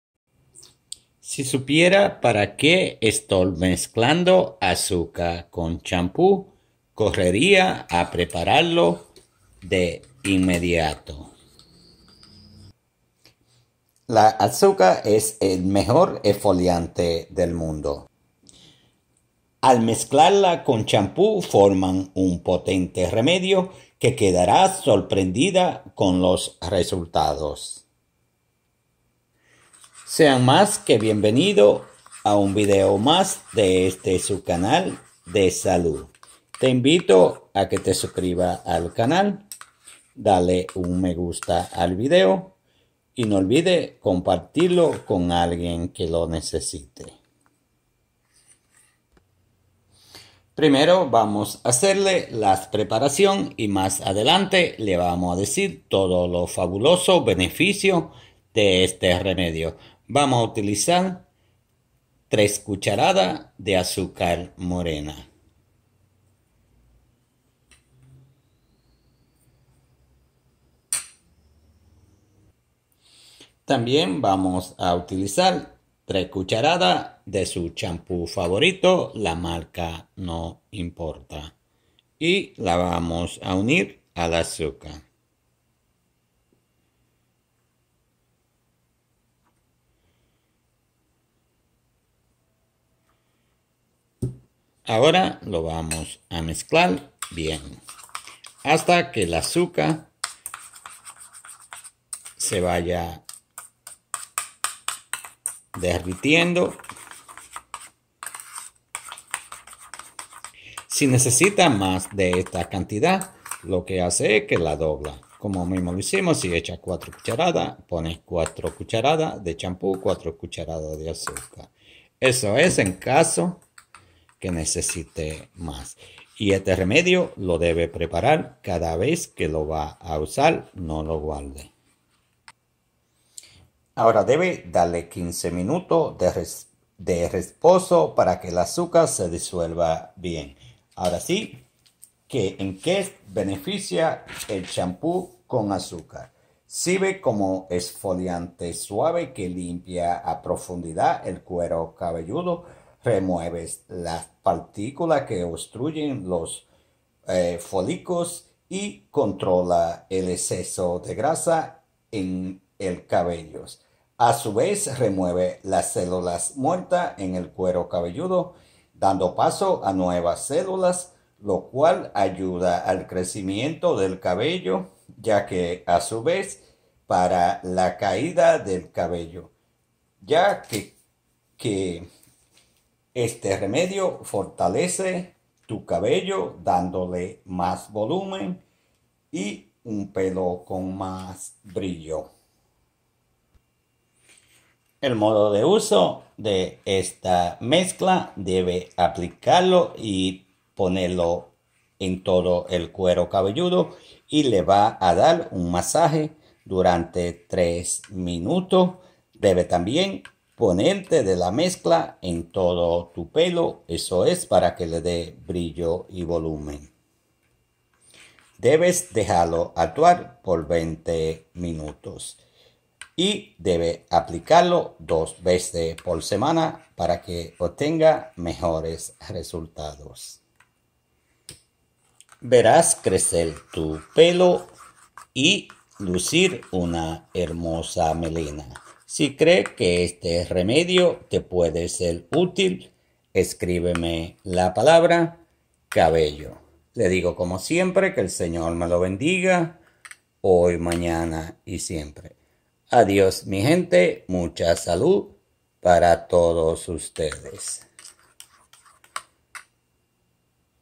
Si Supiera para qué estoy Mezclando Champú con Azúcar!! Correría a prepararlo de inmediato
beneficios del champú con azúcar, para parar la caída del cabello y promover el crecimiento.